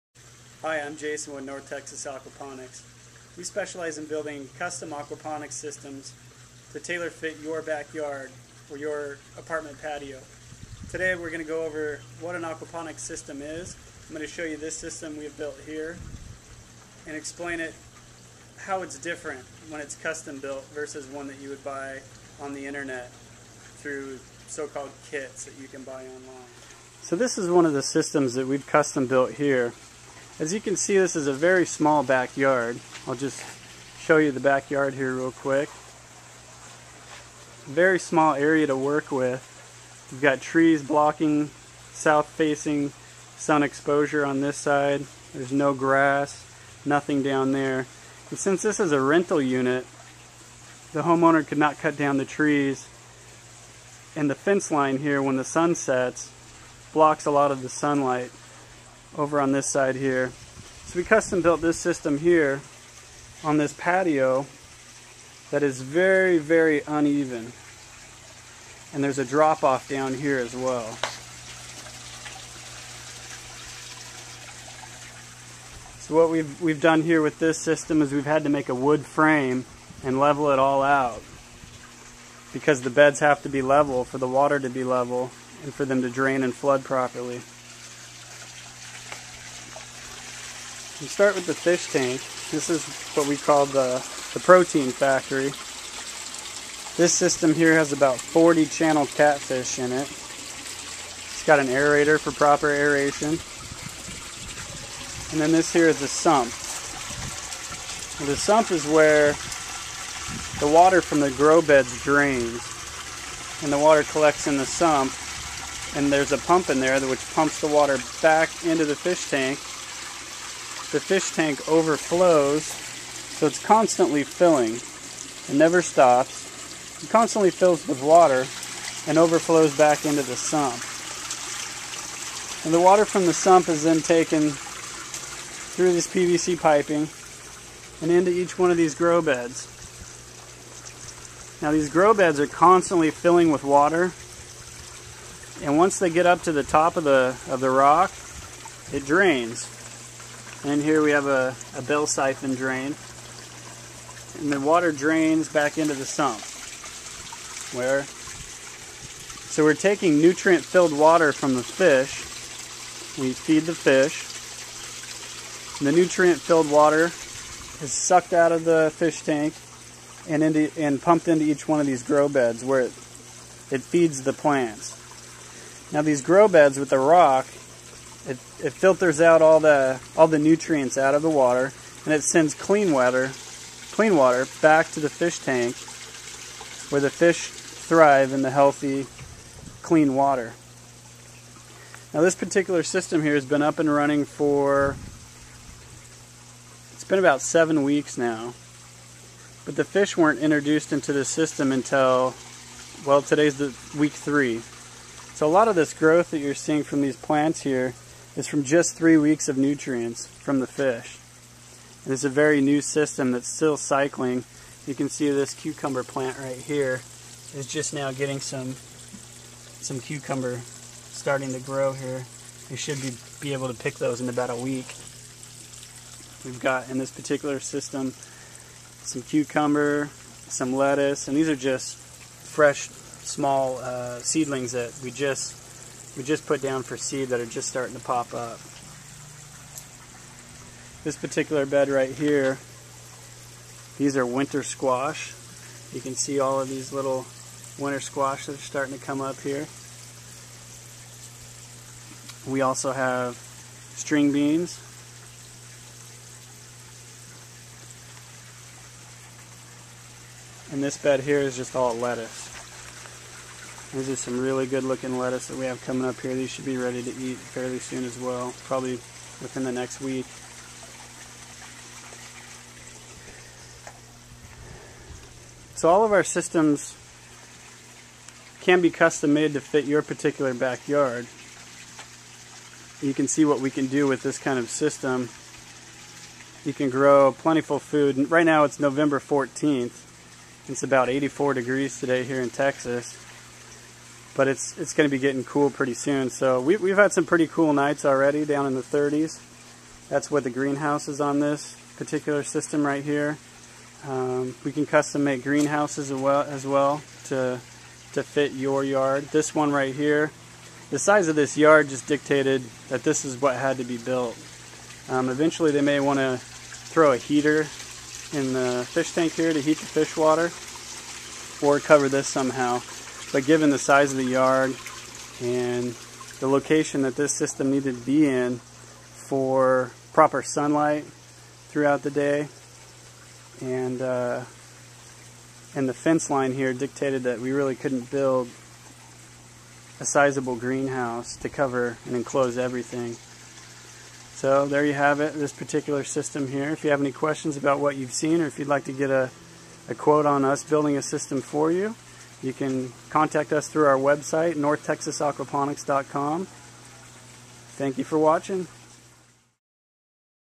Custom Aquaponics build
This video details a custom built Aquaponics system in a very small backyard with difficult building restrictions.